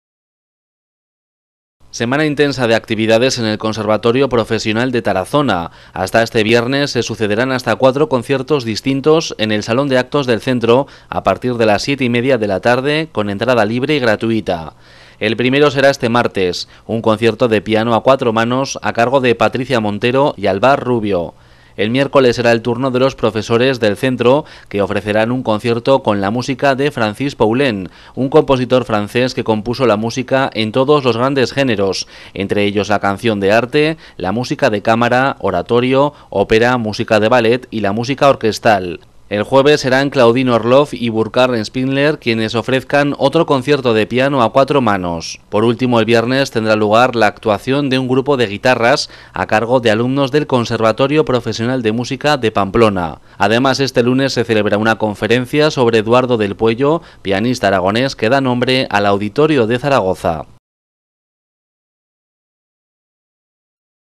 Semana intensa de actividades en el Conservatorio de Tarazona
Semana intensa de actividades en el Conservatorio Profesional de Tarazona. Hasta este viernes se sucederán hasta cuatro conciertos distintos en el salón de actos del centro: piano a cuatro manos, grupo de guitarras y música de Poulenc interpretada por los profesores del centro. Además, hoy se celebra una conferencia sobre Eduardo del Pueyo.